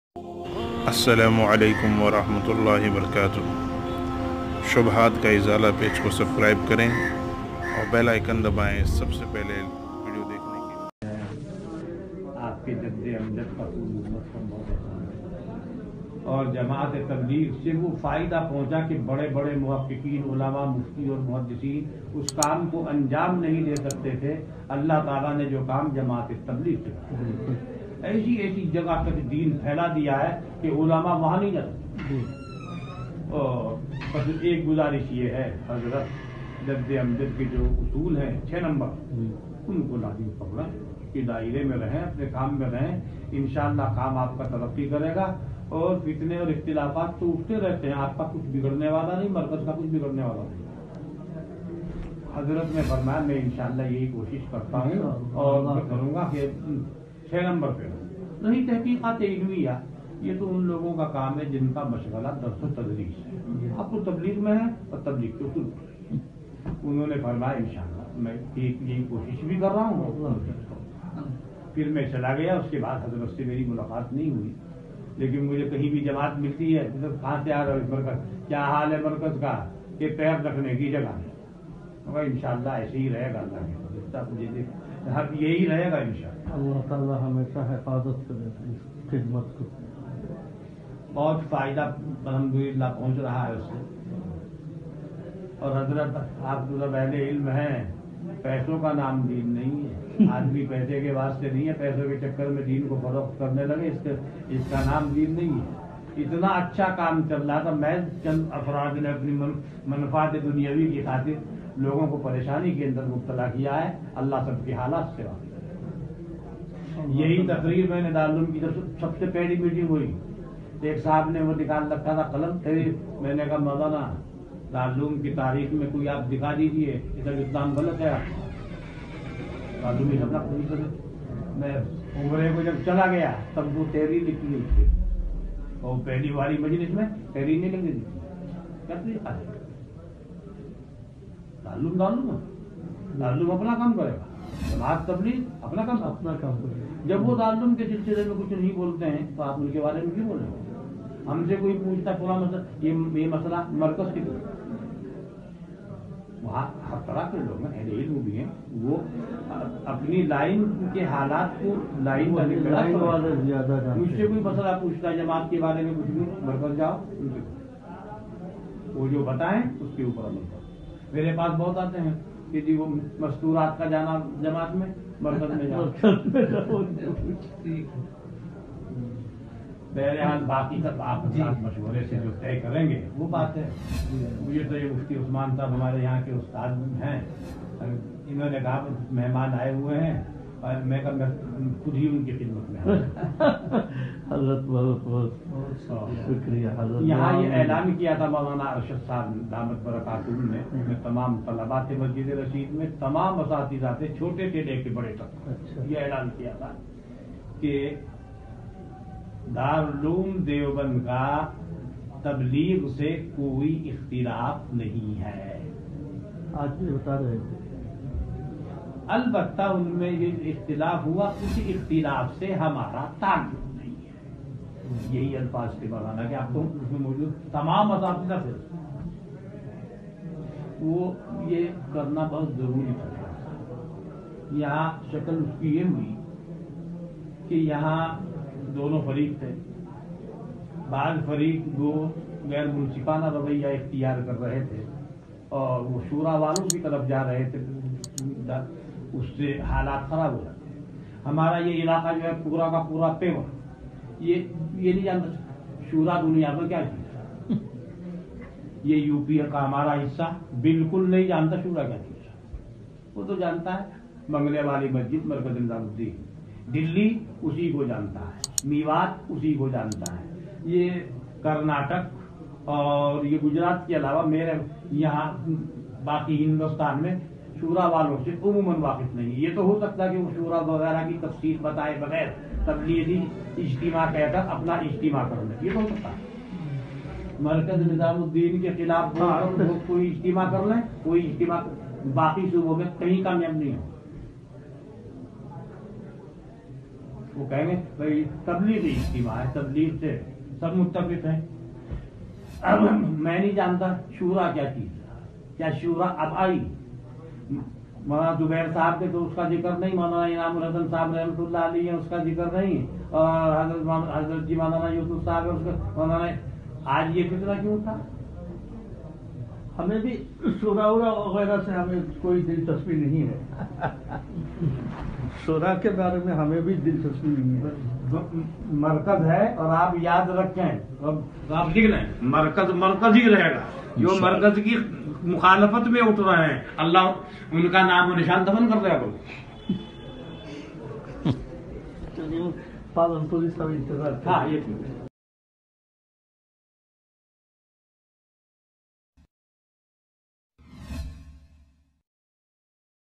Mufti Yusuf Tawali Sahab D.B Ki Aham Wazhaat | Markaz Nizamudin & Hazrat Ji Maulana Sa'ad Sahab
مفتی یوسف تاوْلوی صاحب ہیں ۔فقیہ الامت مفتی محمودحسن گنگوہی رحمتہ اللہ کے خلیفہ ہیں دارالولعلوم دیوبند  کے سابق ناظم تعلیمات و موجودہ استاذ ہیں ۔ دیوبند کے مہمان خانہ میں ایک وفد سے گفتگو کی آڈیو ہے ۔تصدیق شدہ ہے